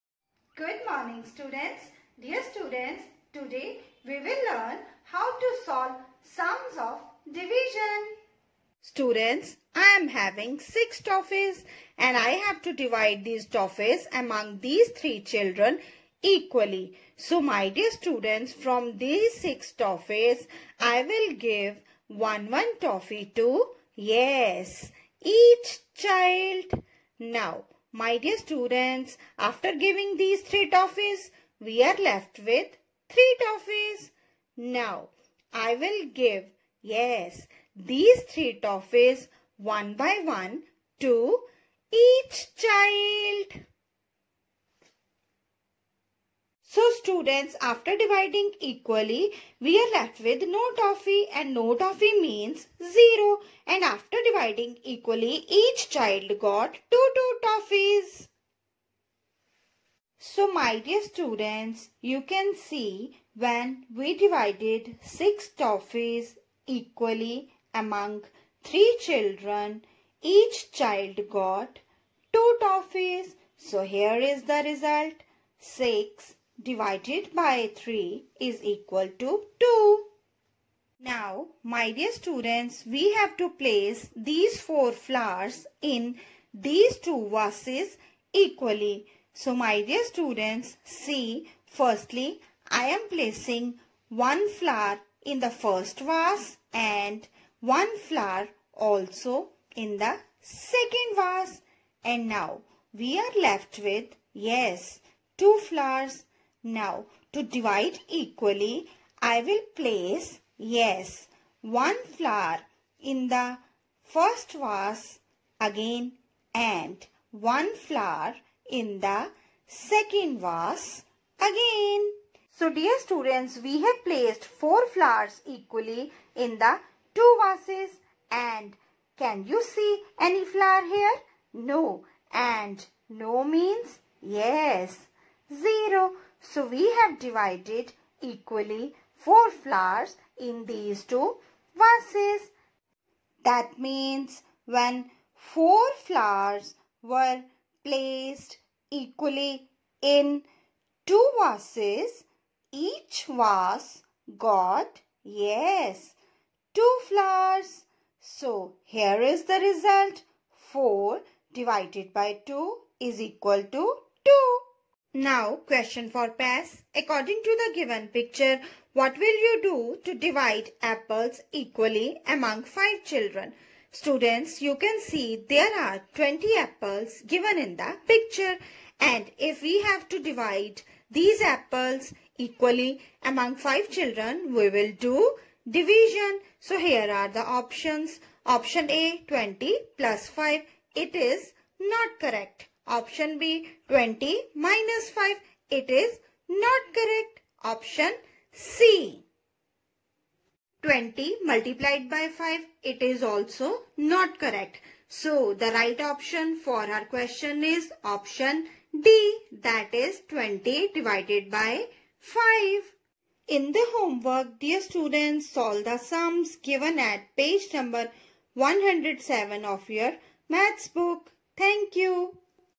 Let's learn how to divide equally in English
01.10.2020

Maths world
Chapter : 4
Page : 107

Let's learn how to divide equally.